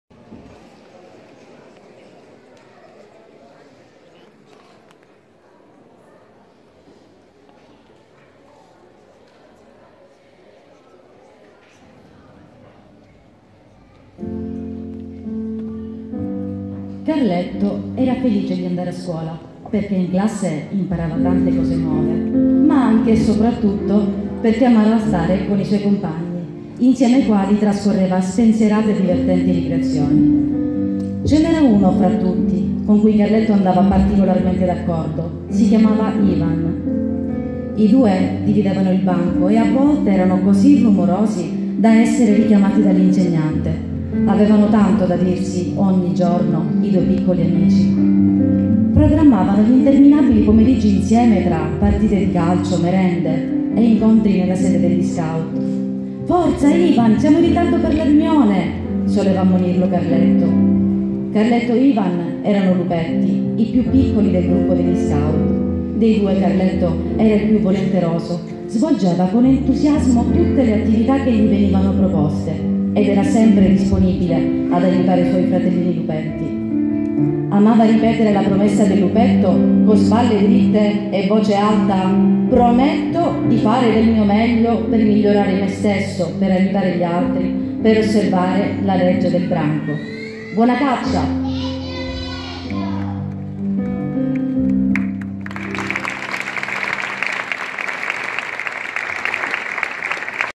Serata per Carlo Perciavalle parte 2
serata per Carlo Perciavalle lettura di alcuni passi tratti dal libro il lupetto Carletto e altre storie